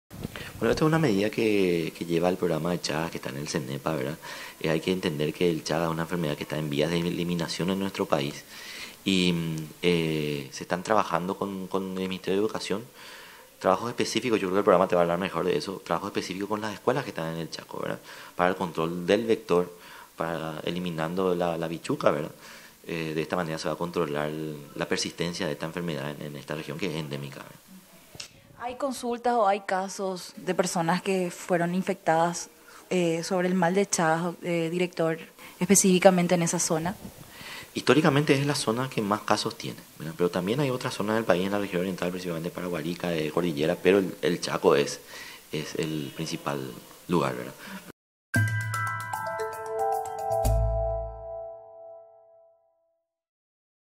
CHACO PARAGUAYO ESCUELAS EN VIGILANCIA ACTIVA LIBRE DE CHAGAS
.Pese a dificultades en lo que va del mes el “Tour 100 escuelas por Chagas” ha visitado 10 escuelas en el Departamento de Presidente Hayes. Beneficiando a niños y adolescentes de 5 a 15 años.